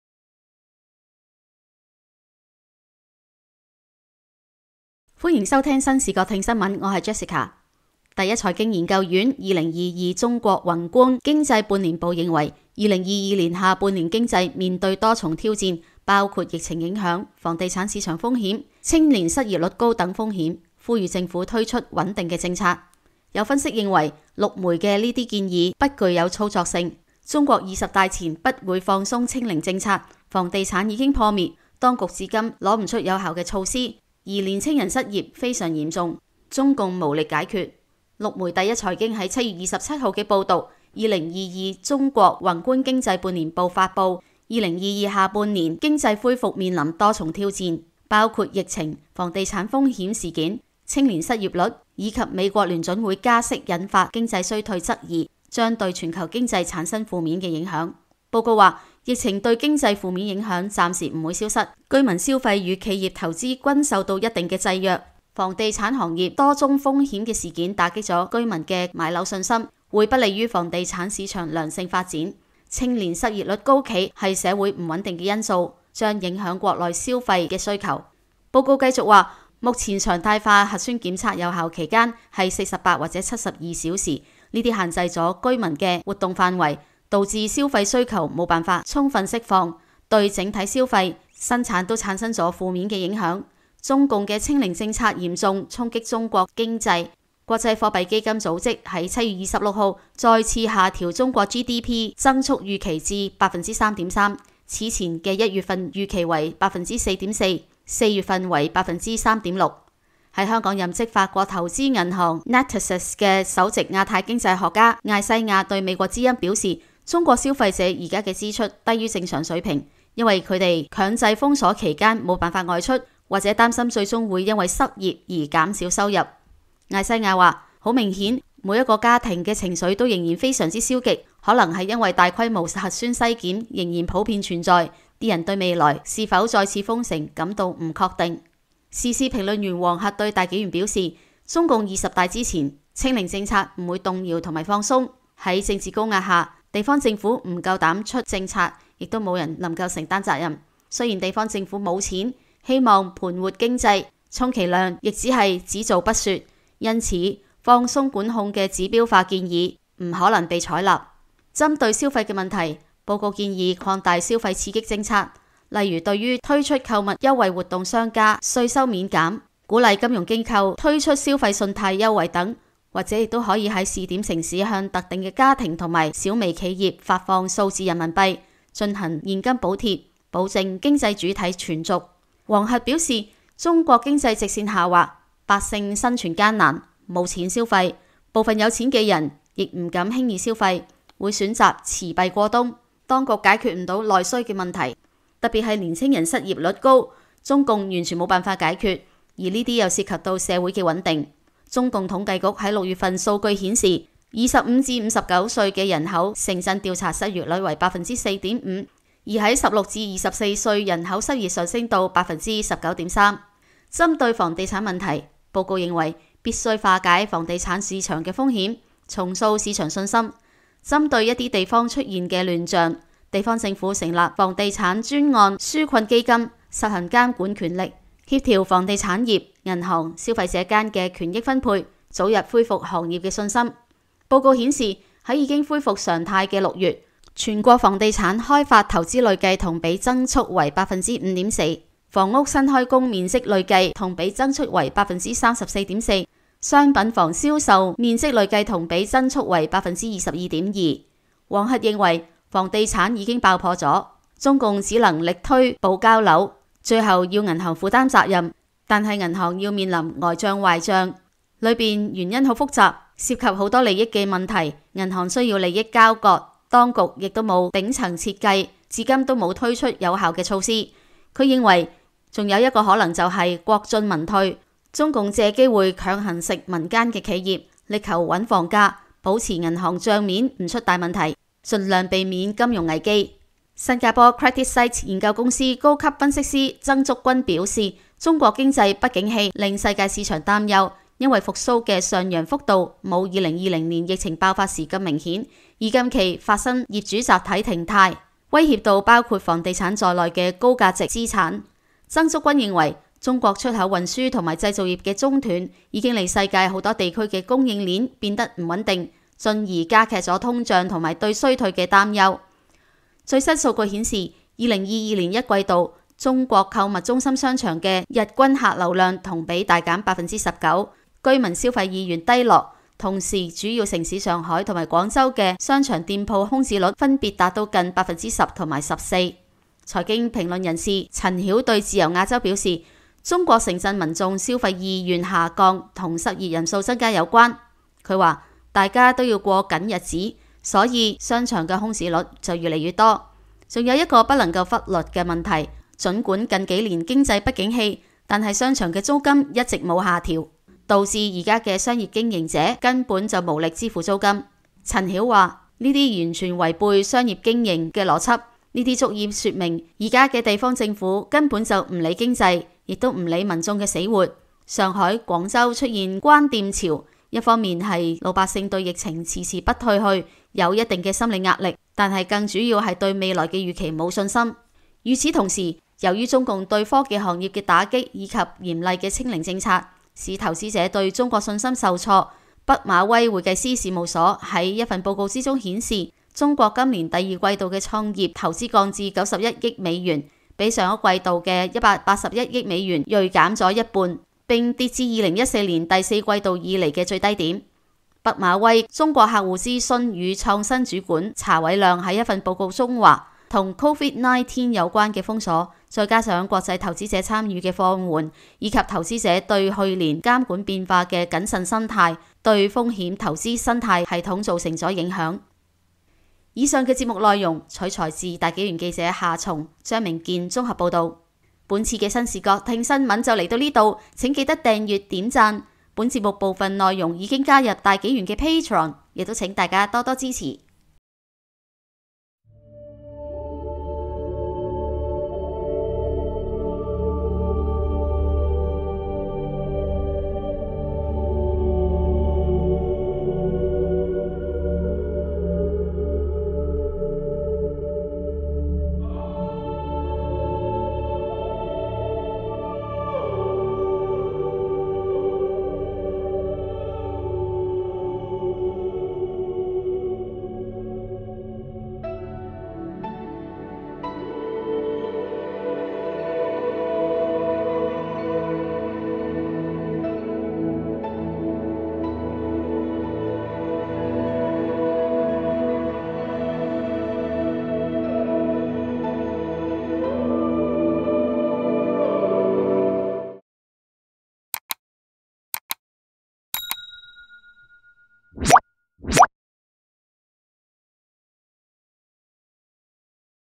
【新視角聽新聞】第一財經研究院《2022中國宏觀經濟半年報》認為，2022下半年經濟面對多重挑戰，即疫情影響、房地產市場風險、青年失業率高等，呼籲政府推出穩定政策。
新紀元７月刊【清零與經濟的拉鋸】：📖現已上市🎉